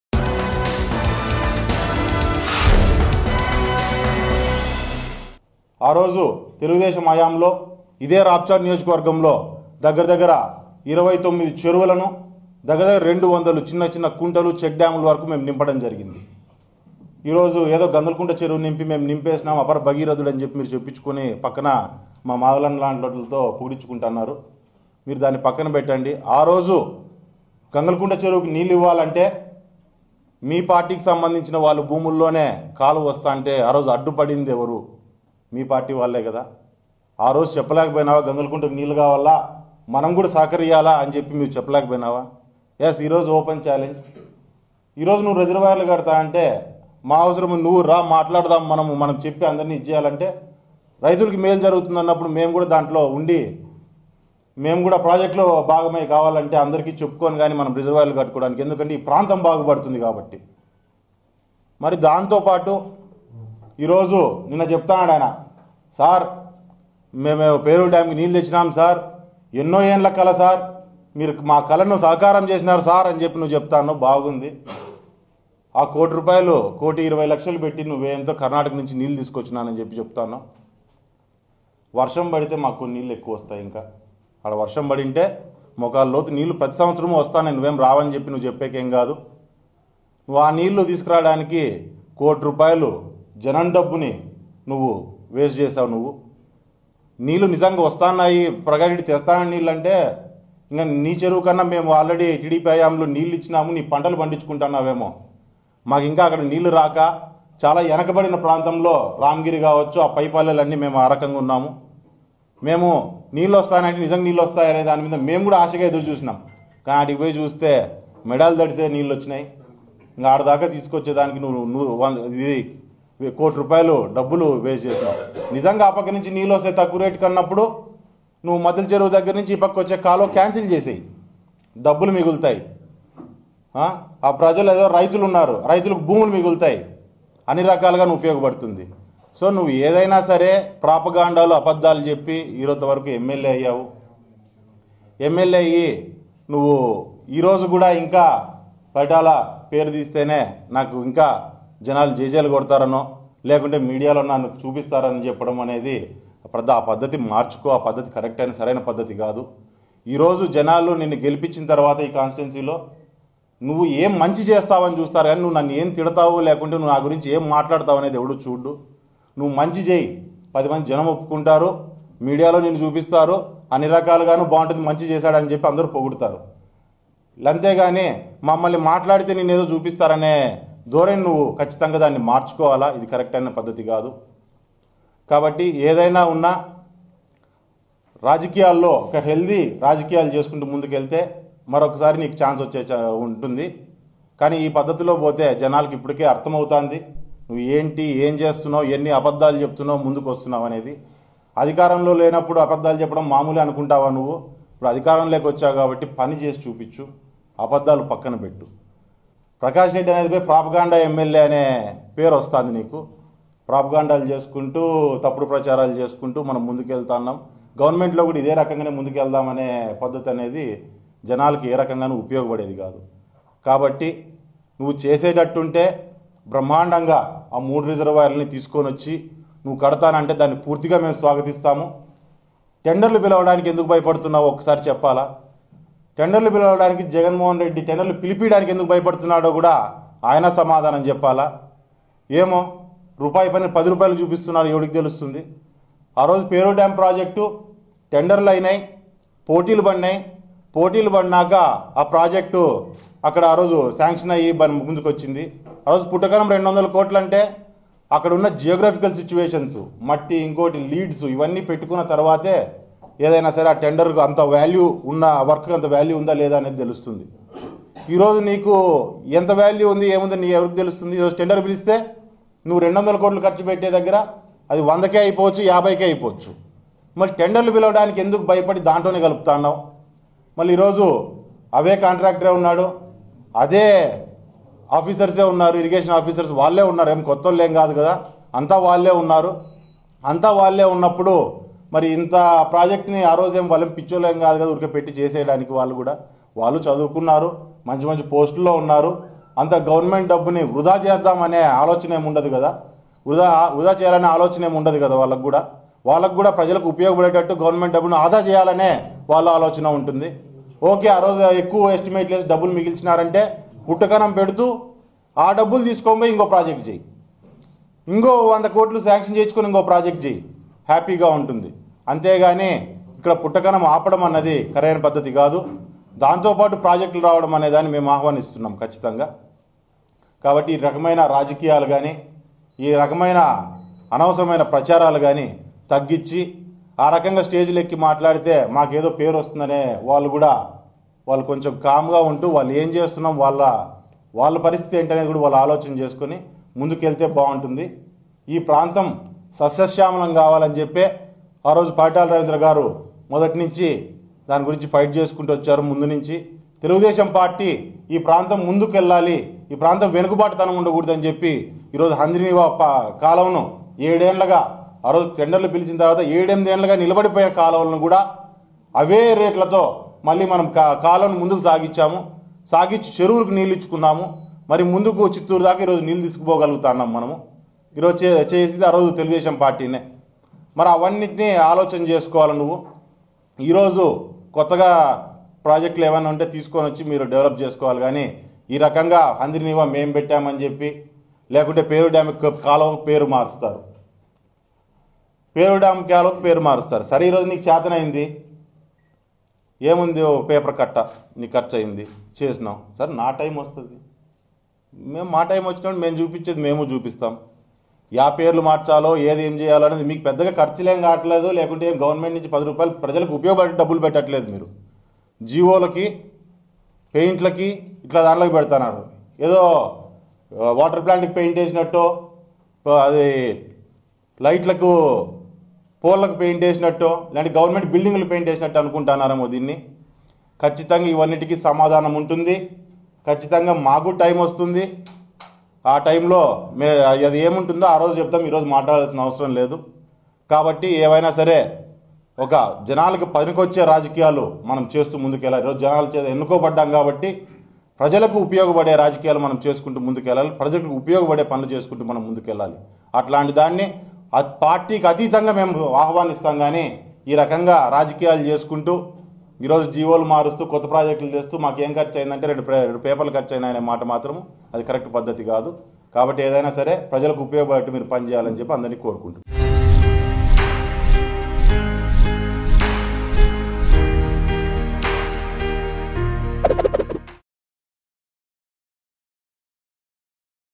TDP Leader Paritala Sriram Gives Serious Warning In Press Meet | AP Latest Updates | Mango News
TDP Leader Paritala Sriram Gives Serious Warning In Press Meet.